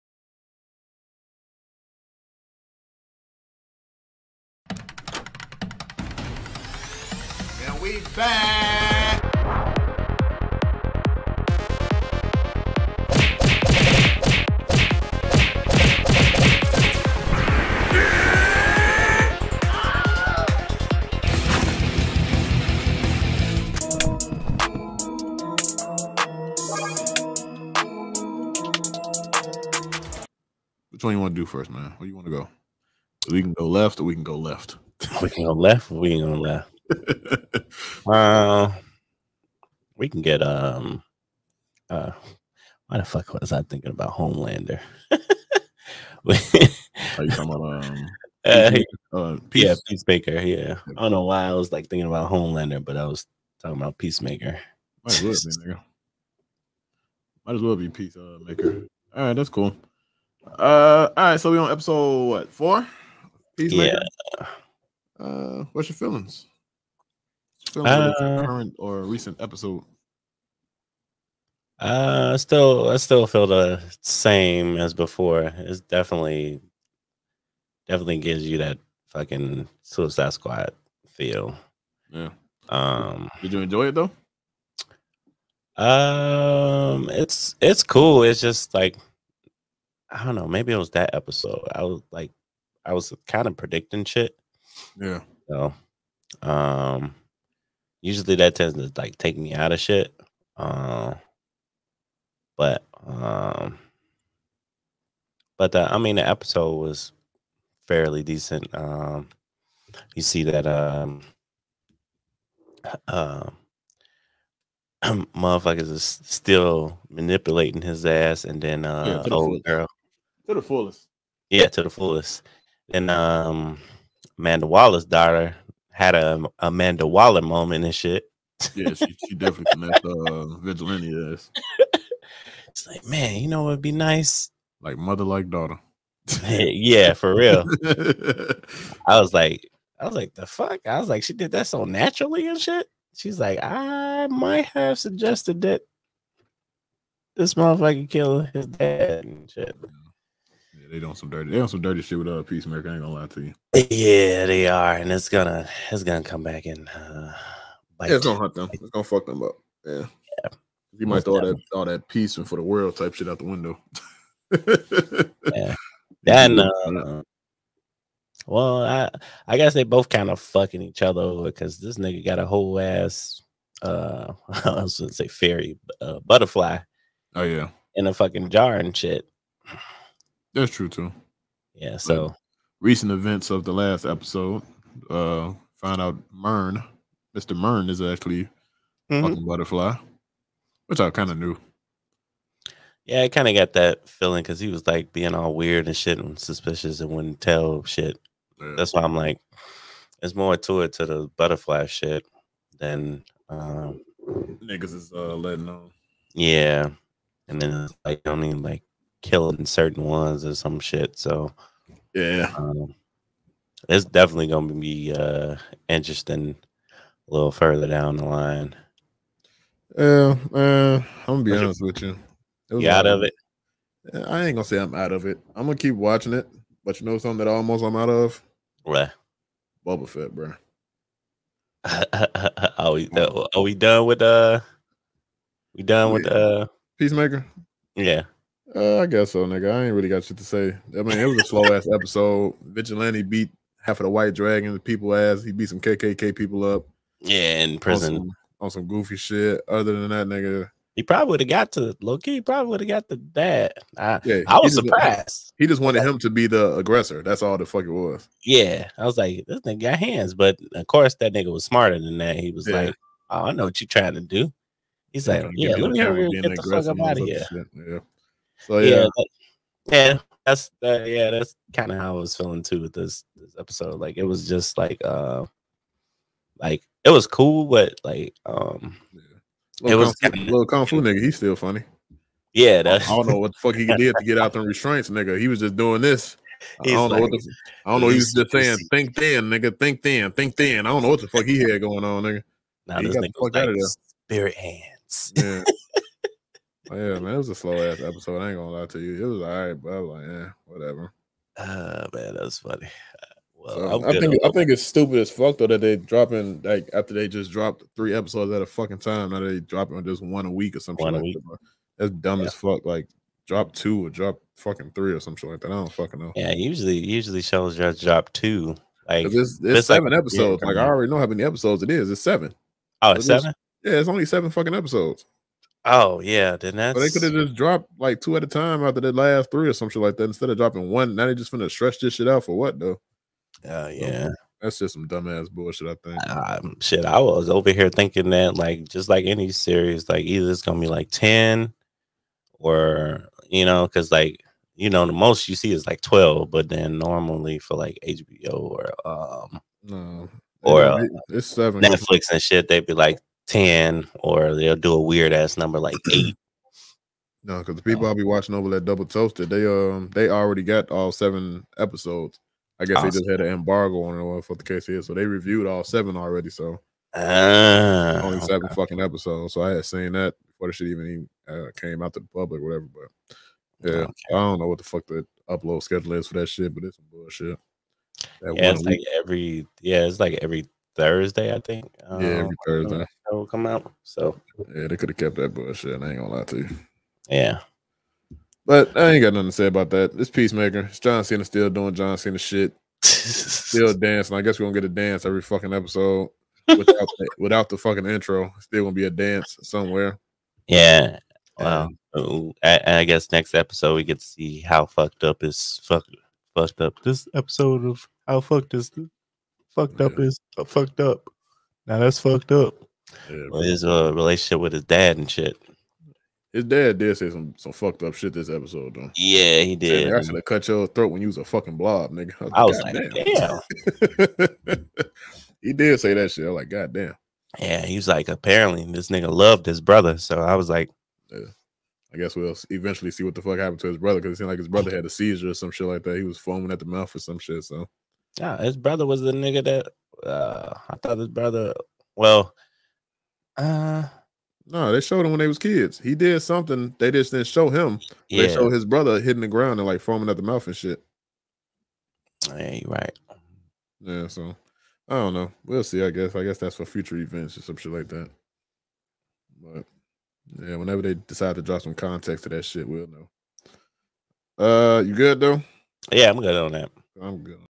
Peacemaker Ep. 4 Review and Spoiler Talk | DREAD DADS PODCAST | Rants, Reviews, Reactions
Original Video:
Background Art By:concrete_381

2 Dads with Dreads giving thoughts and opinions on weekly current and popular events.